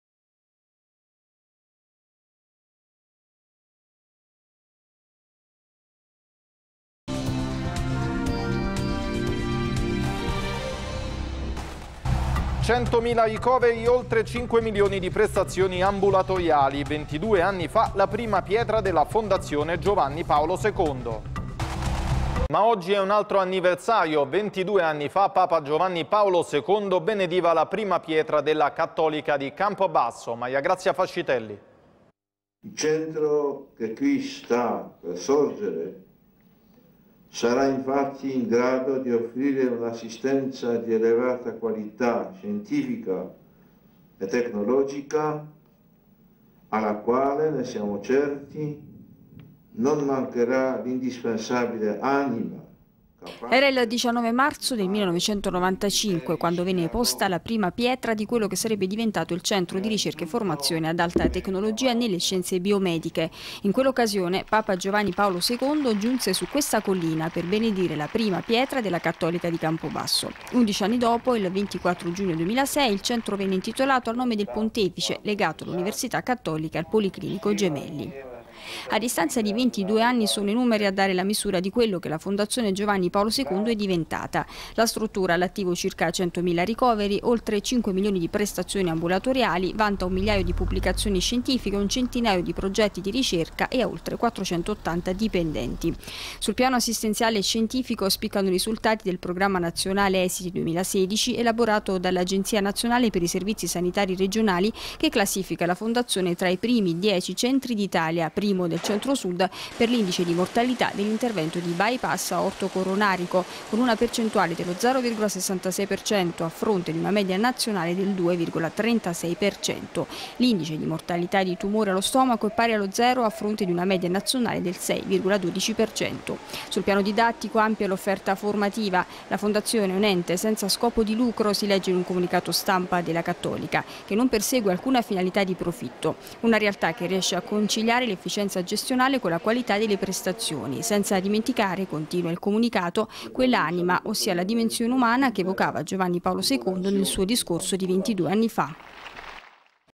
VENTIDUE ANNI DALLA POSA PRIMA BENEDETTA DA SAN GIOVANNI PAOLO II
Pubblicato il 20 mar 2017

L'INIZIO DI UN CAMMINO RICCO DI SUCCESSI. Il 19 marzo è una data memorabile per il Molise: ventidue anni fa, il 19 marzo 1995, venne infatti posta la prima pietra di quello che sarebbe diventato il Centro di Ricerche e Formazione ad Alta Tecnologia nelle Scienze Biomediche. In quell’occasione Giovanni Paolo II giunse su questa collina per benedire la prima pietra del Centro.
Undici anni dopo, il 24 giugno 2006 il Centro venne solennemente intitolato al nome del Pontefice, così vicino e affettuosamente legato all’ Università Cattolica.
La Fondazione “Giovanni Paolo II” è un avamposto in ambito assistenziale, in quello degli avanzamenti scientifici e tecnologici, in quello, anche, dell’alta formazione e della comunicazione relativi alle sempre più estese aree di attrazione della medicina. La struttura ha all'attivo circa 100 mila ricoveri, circa 5 milioni di prestazioni ambulatoriali, vanta un migliaio di pubblicazioni scientifiche e un centinaio di progetti di ricerca. La Fondazione è un Ente senza scopo di lucro che non persegue alcuna finalità di profitto, una realtà che riesce a conciliare l’efficienza gestionale con la qualità delle prestazioni erogate: i conti sono in ordine ed è stata raggiunta la necessaria solidità finanziaria per continuare ad offrire servizi di qualità alla popolazione molisana e non. L’avanzamento tecnologico è una delle finalità principali, con il costante aggiornamento delle apparecchiature in dotazione